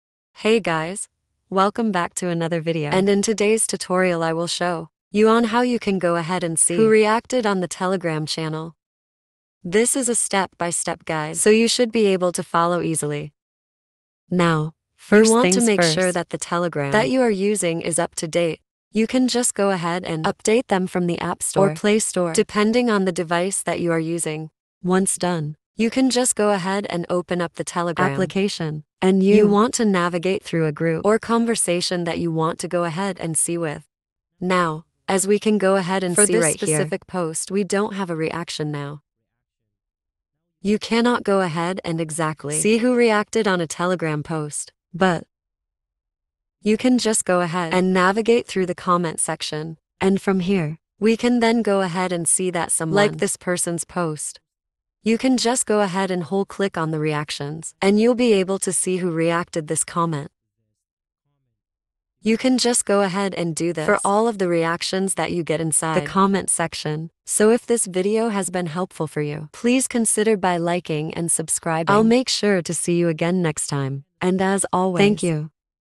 How to See Who Reacted on Telegram Channel
This video tutorial provides a step-by-step guide on how to see who reacted to messages in your Telegram channel. It covers navigating the channel's settings, accessing message details, and viewing the list of users who have reacted. Whether you're managing a large community or simply want to track engagement, this video ensures you can easily see who reacted on your Telegram channel.